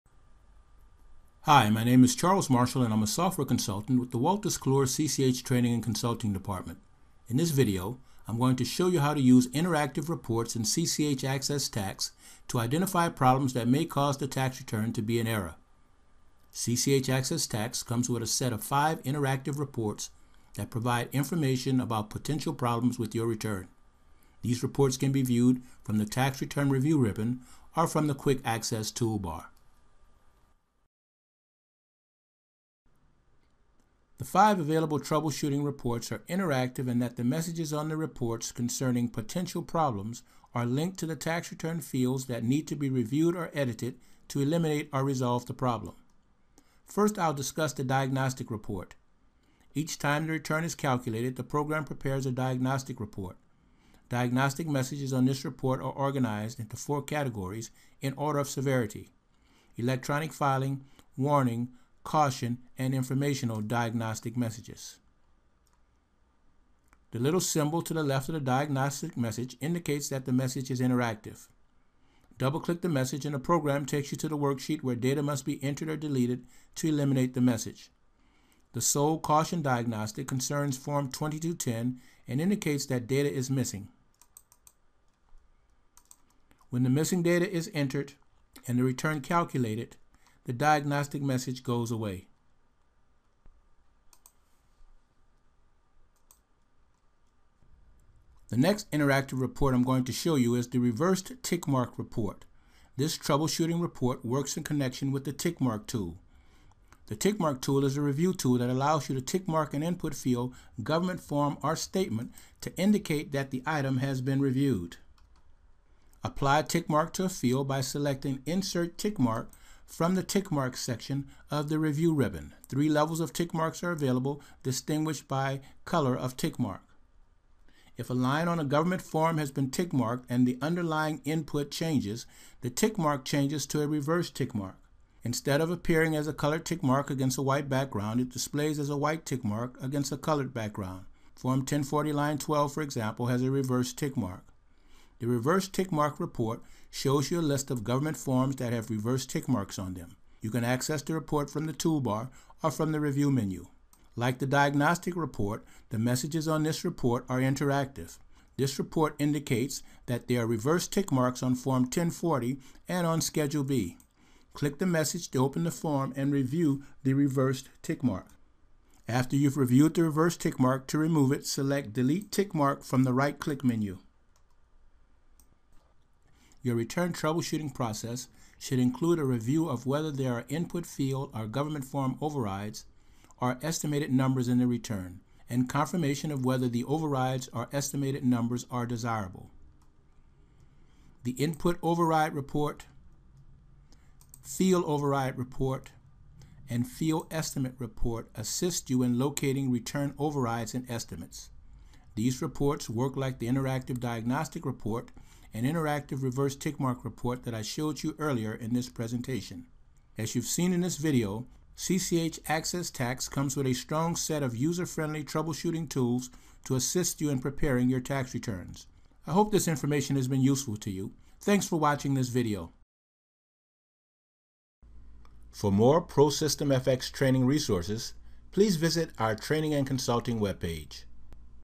CCH Axcess™ Tax - Using Interactive Reports
Master the use of the Interactive Reports available in CCH Axcess™ Tax. Learn about additional training options by visiting